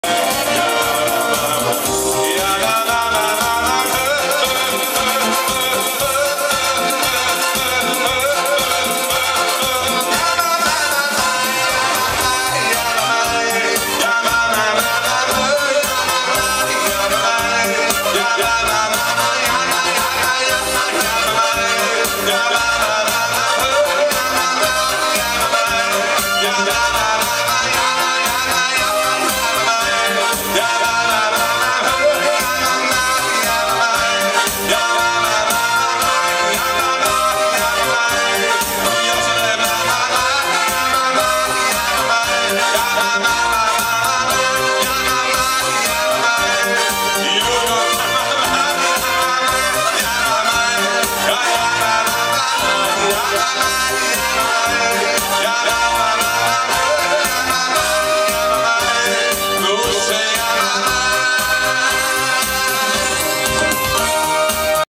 Greatest Chassidish Singer of all time, Michoel Schnitzler!
The one and only Michoel Schnitzler singing at a lebedige chassene, his great talent and beautiful voice, together with Pinny Ostreichers jingeling fingers on the keyboard, enhanced the chassene and the spirits were uplifting while the oilem was dacing till the early morning hours...